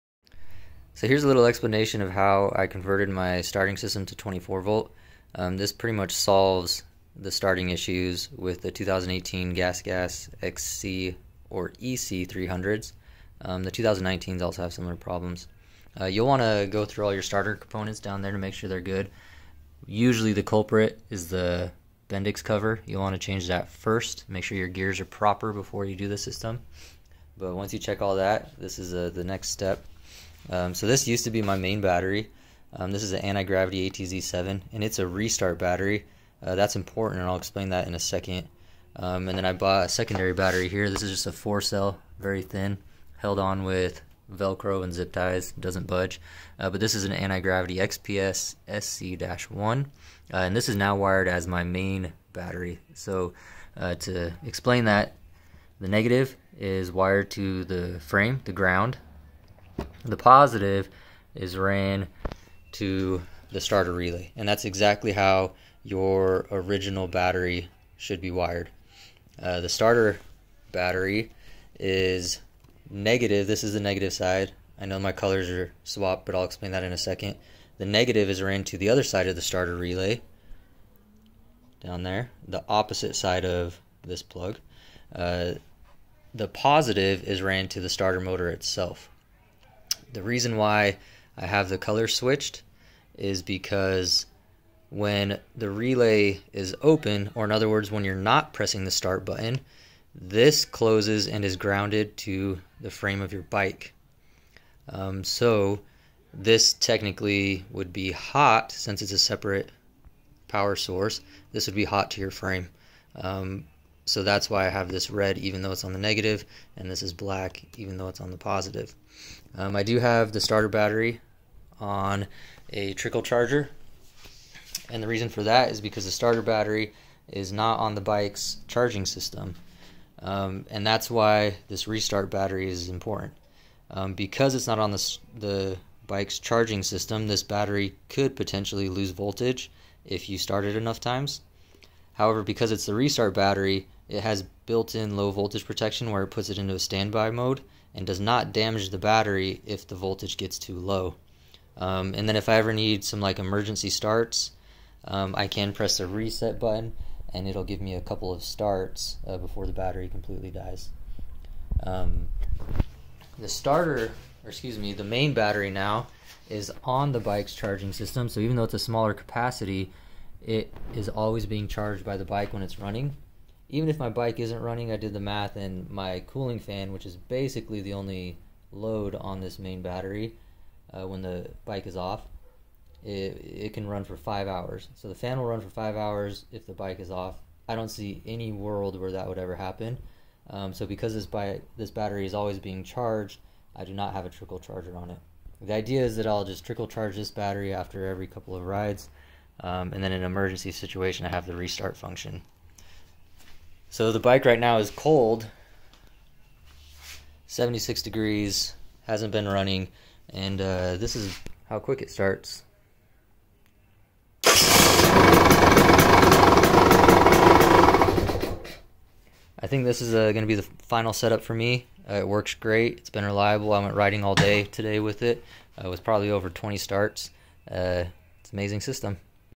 Dirt bike starter Fix - 24 volt system
This 24V system could be adapted to almost any dirt bike model. However, this video specifically applies to 2018 and 2019 Gas Gas models as well as 2021 and 2022 Rieju models.

Antigravity ATZ-7 Restart battery
Antigravity 4Cell Battery
Alternative small battery - Firepower Featherweight

Please see the Thumpertalk Forum for guidance on why your system may not be working.

The 24V starter system is the LAST step. Do not do this if you have not gone through all the issues outlined in the above Forum.

Sending 24v to your starter is a very simple setup but please attempt at your own risk.